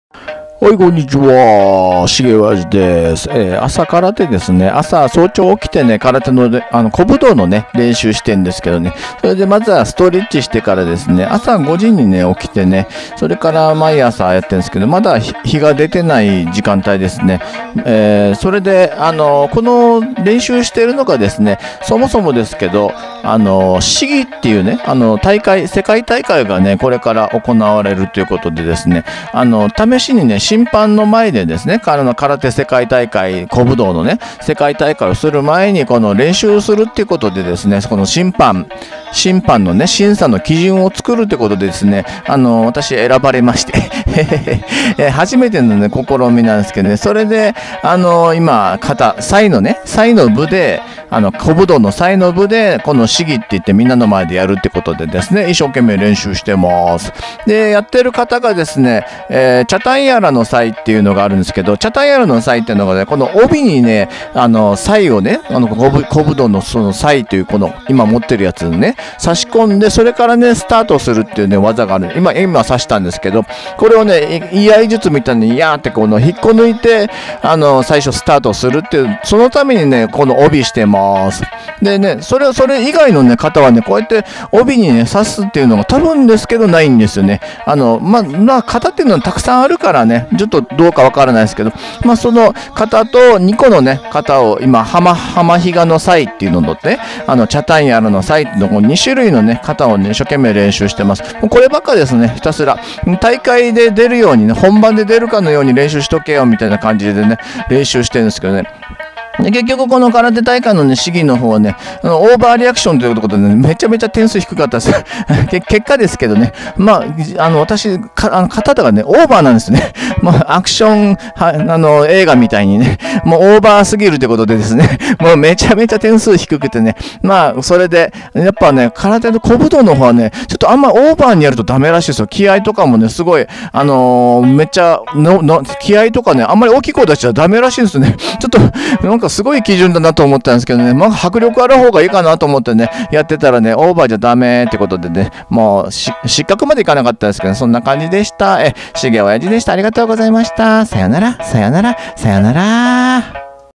【家道場】沖縄空手世界大会の試技の練習 古武道サイ 沖縄空手世界大会 OKINAWA KARATE WORLD TOURNAMENT
☆しげおやじのYouTubeチャンネルは全部で５チャンネル

空手のルーツ沖縄で空手古武道の練習をしているモーニングルーティーン動画です。

☆サイ☆　カタの名前

「チャタンヤラノサイ」
「ハマヒガノサイ」